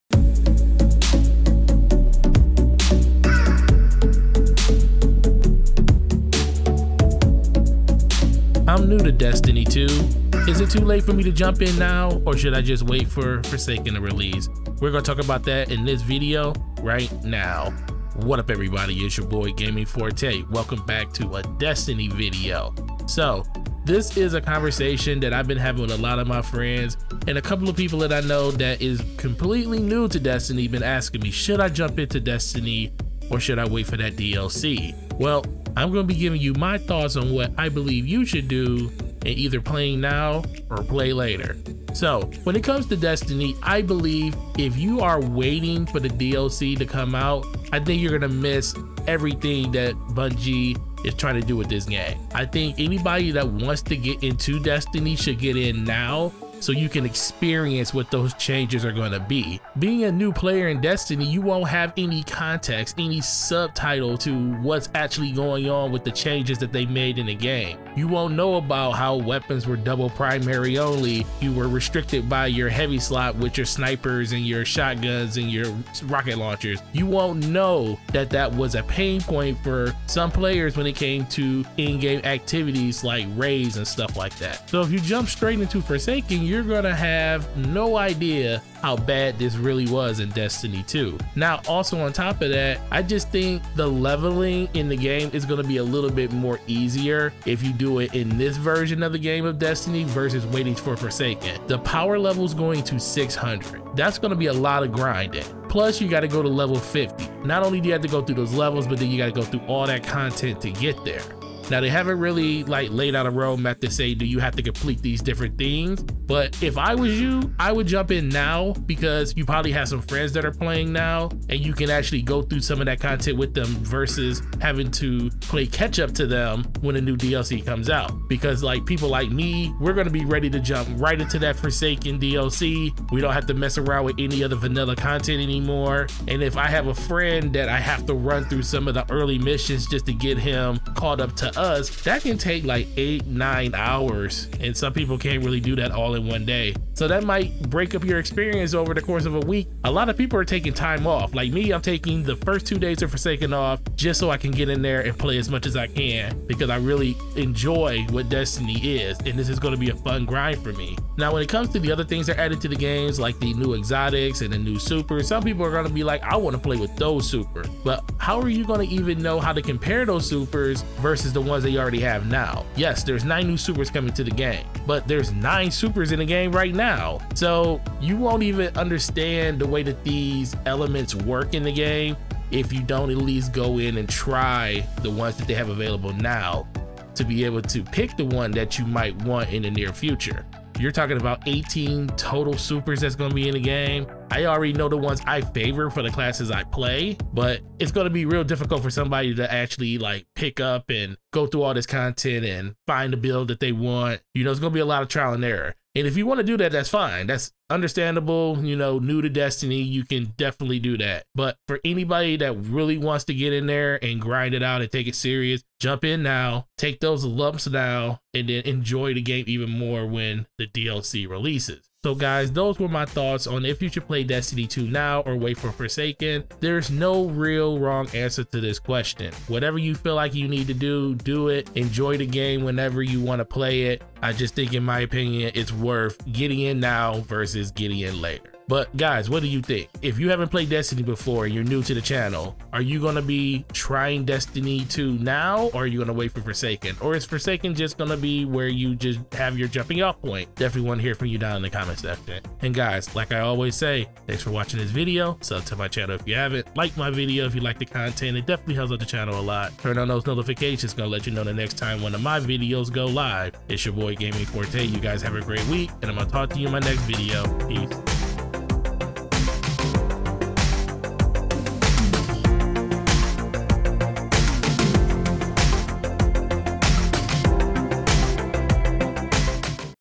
Why you should play Destiny 2 NOW before the release of Forsake
I talk about the reason I believe you should play Destiny 2 now before the release of the Forsaken DLC.

If you need a good group of guy to play some of the best games with don't look any further.